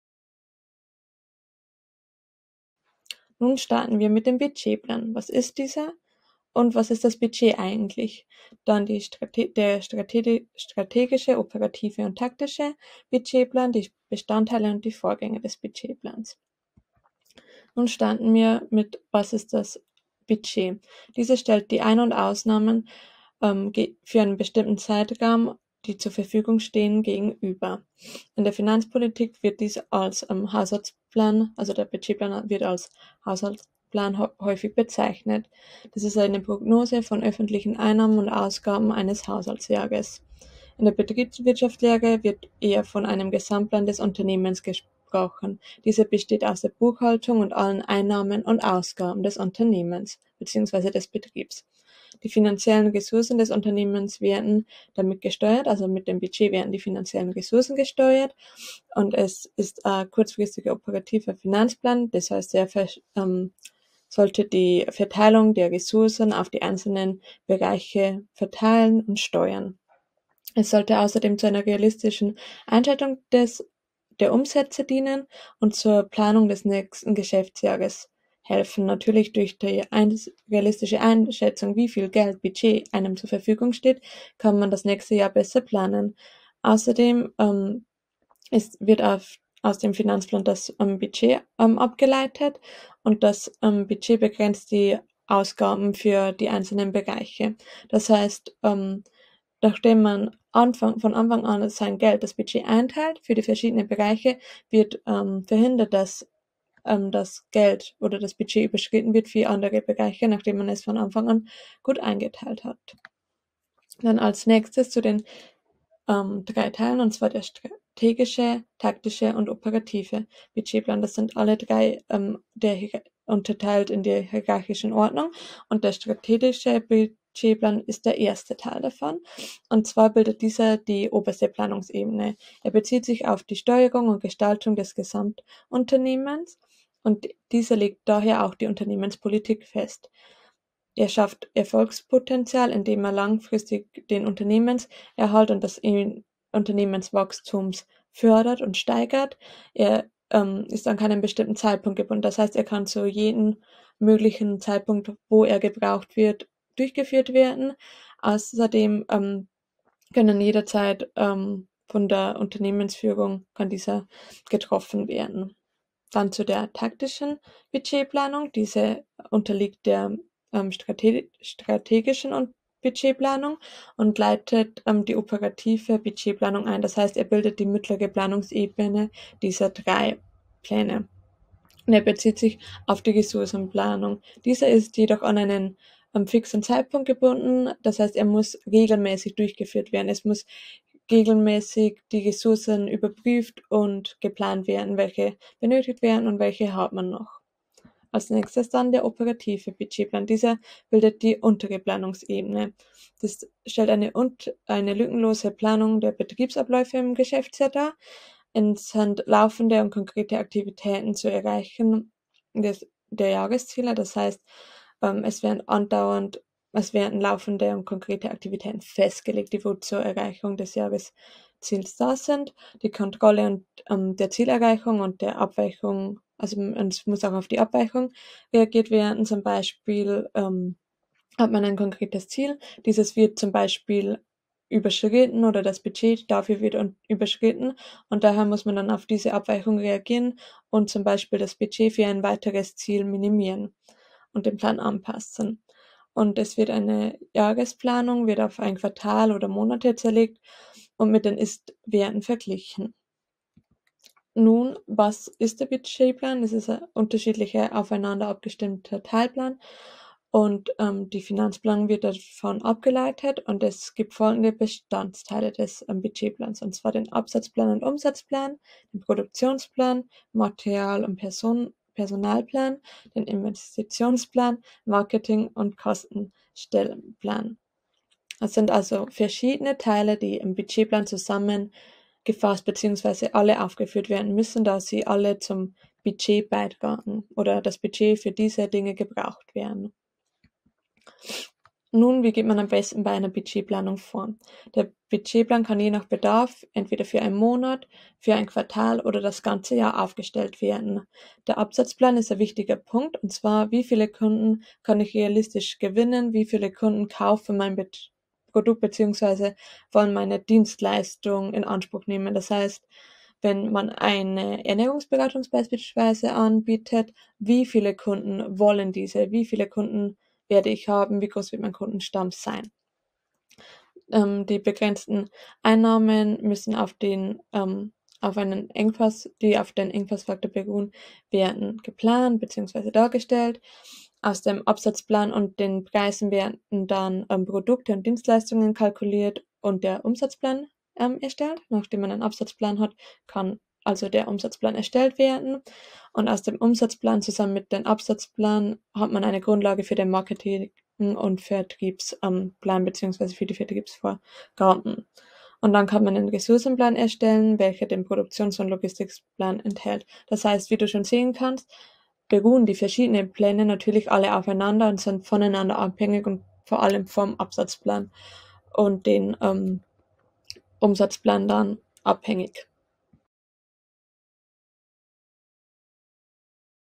Selbstständig Arbeiten im Gesundheitssektor | Was sind das Budget und der Budgetplan?
*Thema:* Budget & Budgetplan
*Kurs:* Selbstständig Arbeiten im Gesundheitssektor - Unternehmensgründung in der Praxis | Wissensvideo 15
*Dozent*in:* Vanessa Praml

► 00:00 Intro
► 00:17 Was ist das Budget und der Budgetplan?
► 01:54 Strategische Budgetplanung
► 02:47 Taktische Budgetplanung
► 03:18 Operative Budgetplanung
► 04:13 Bestandteile des Budgetplans
► 04:51 Wie gehe ich bei der Budgetplanung vor?

📖 *Deine Vorteile*
▸ Förderungen möglich